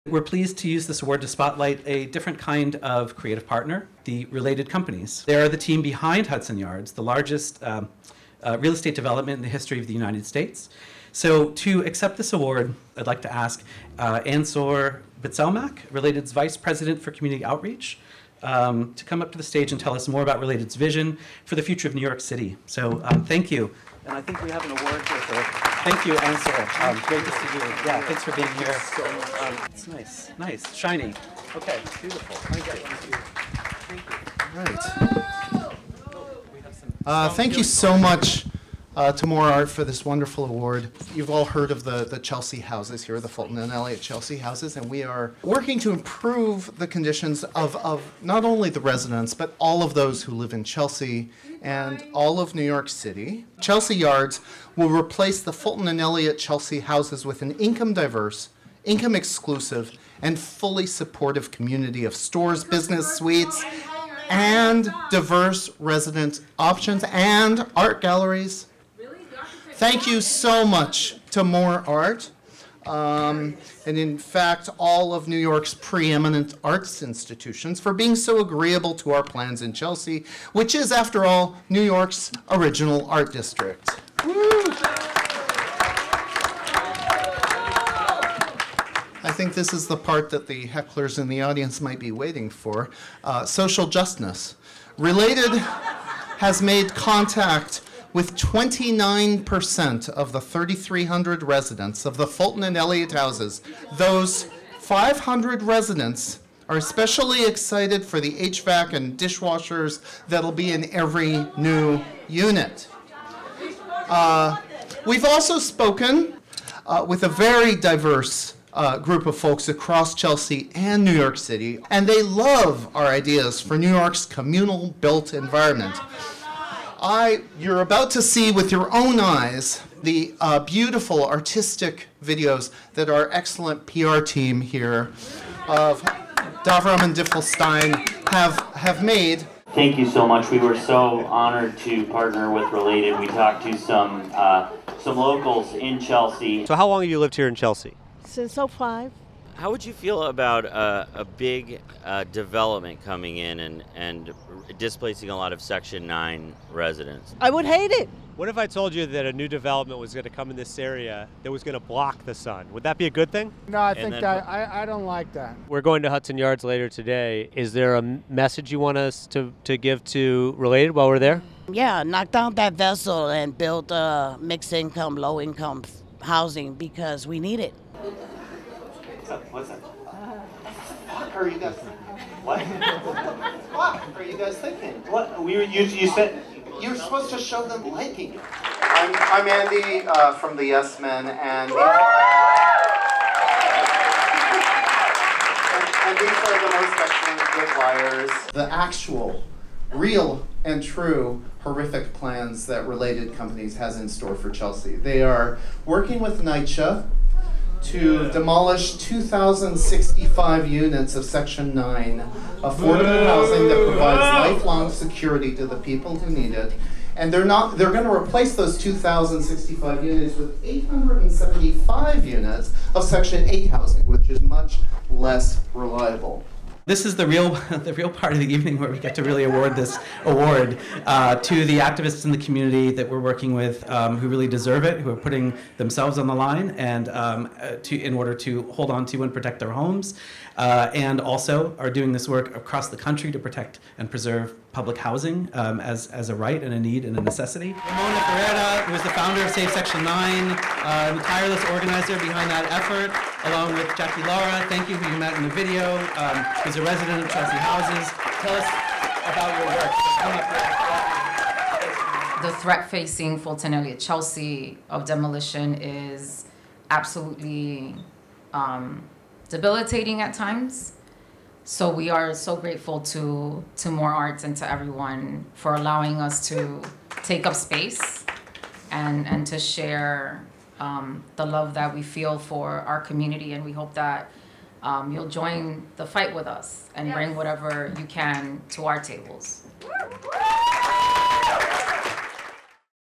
Related Companies announce demo of public housing in Chelsea
New York and developers are conspiring to destroy New York's most important asset. for full story.